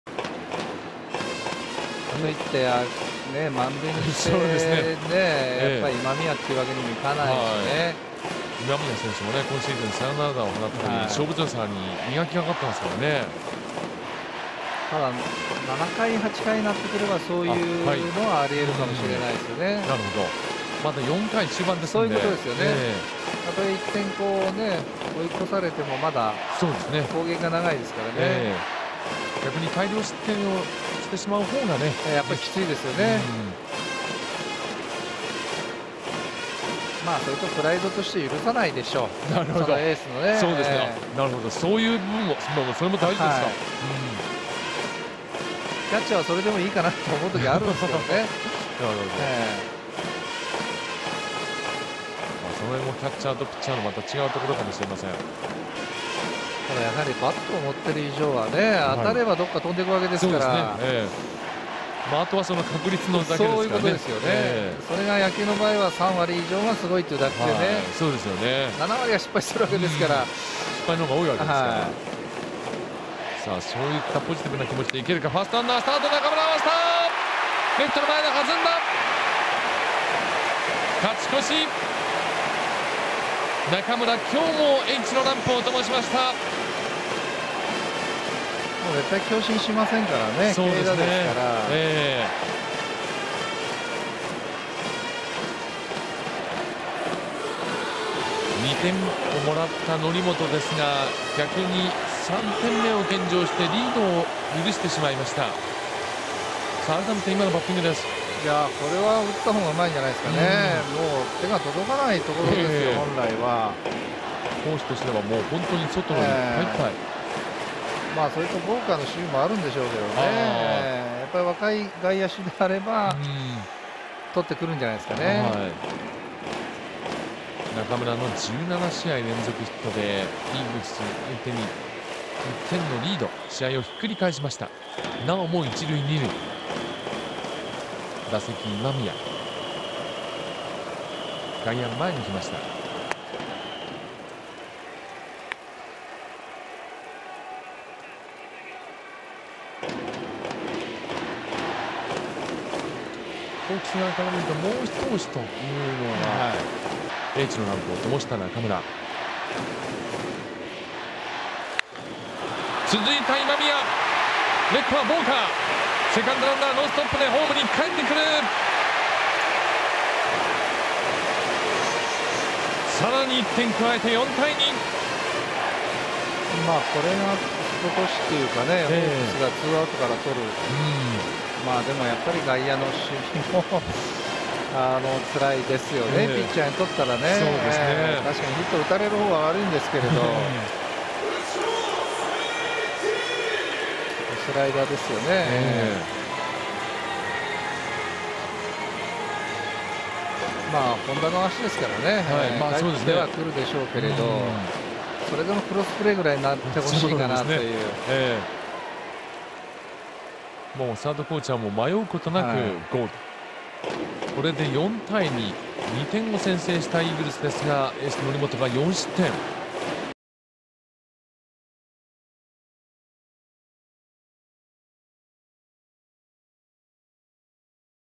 【プロ野球パ】若き1・2番、17試合連続安打の中村＆今宮の連続適時打 2014/07/05 H-E
4回、ソフトバンクの若き1・2番コンビ、中村と今宮が連続してタイムリーを放ち、勝ち越し。 中村はこのタイムリーで17試合連続安打。（7月5日 ソフトバンク-楽天）